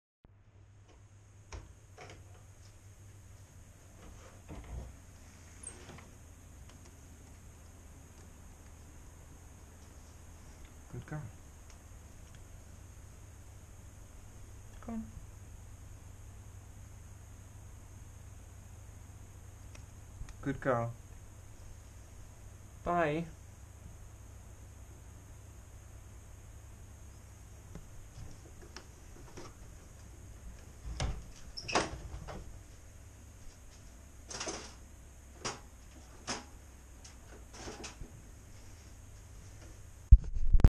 Fox Is Now Eating of My Hand
Friendly Fox in my garden, I'm actually quite worried about her because I haven't seen her for a few days.